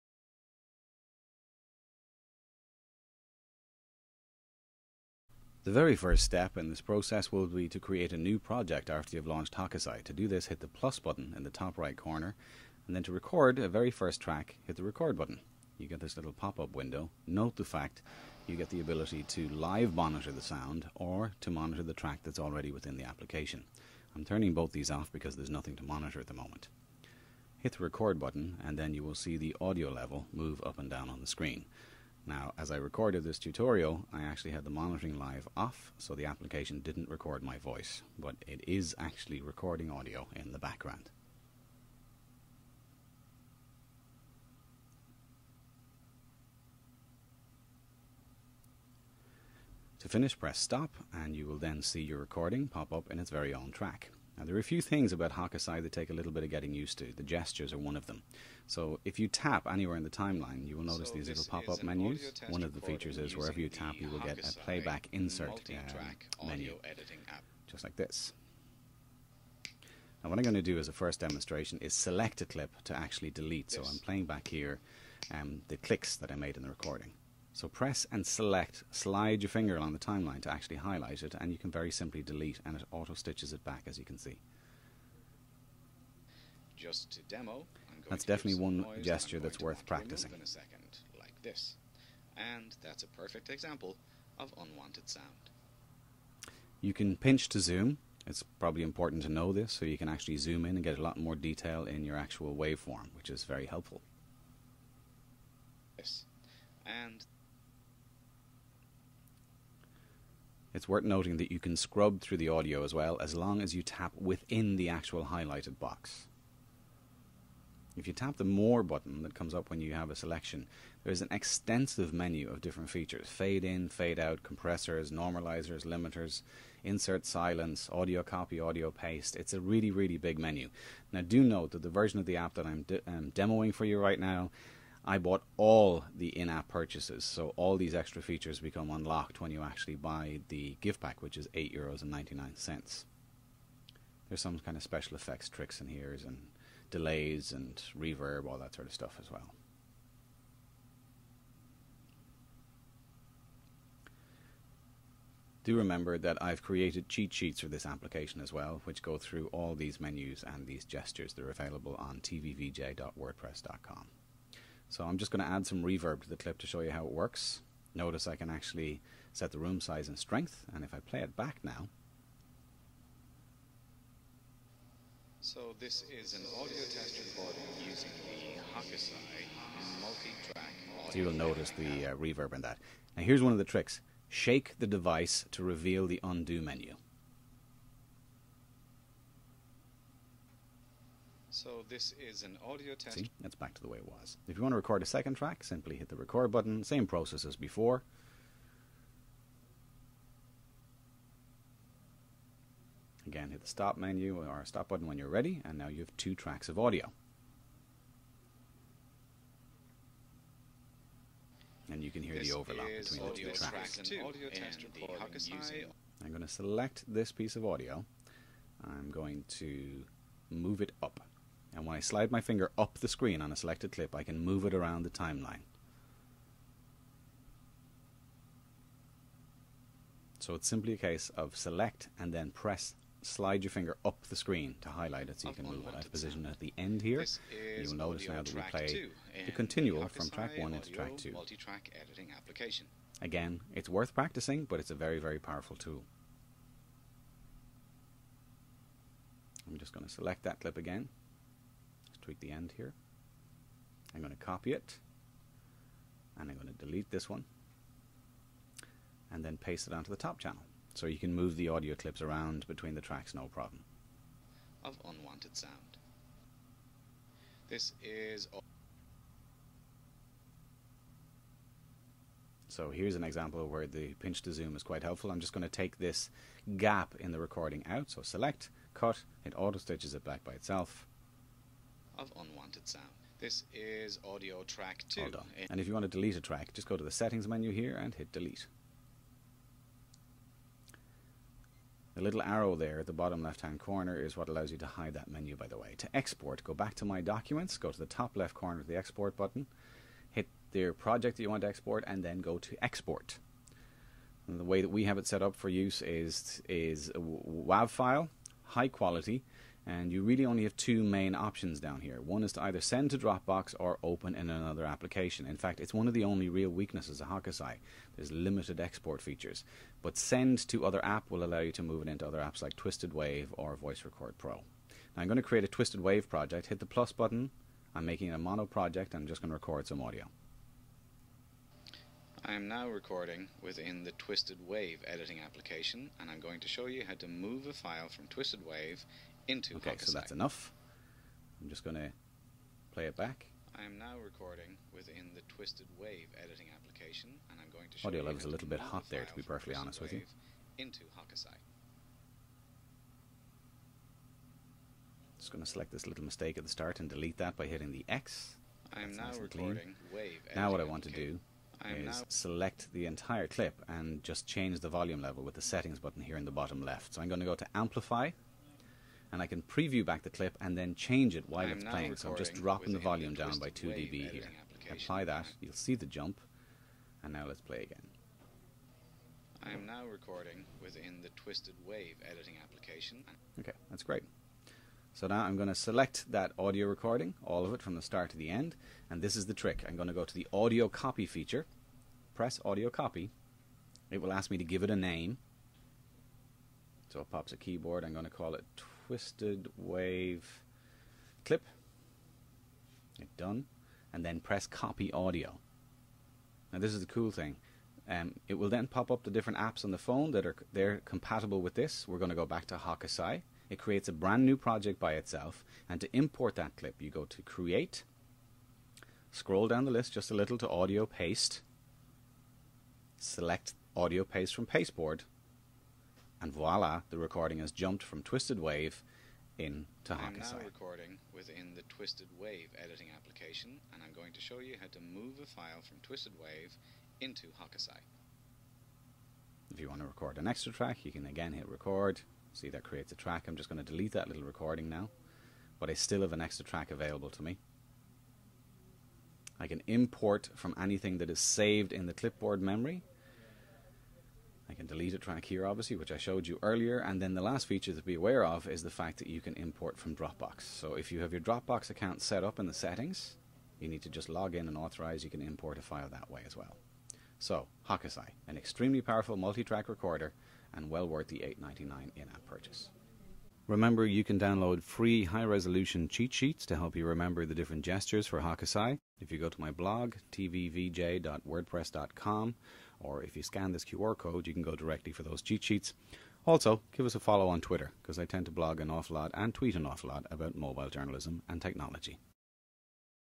HOKUSAI Overview
A brief overview of the Multi-Track audio editing application for iOS: Hokusai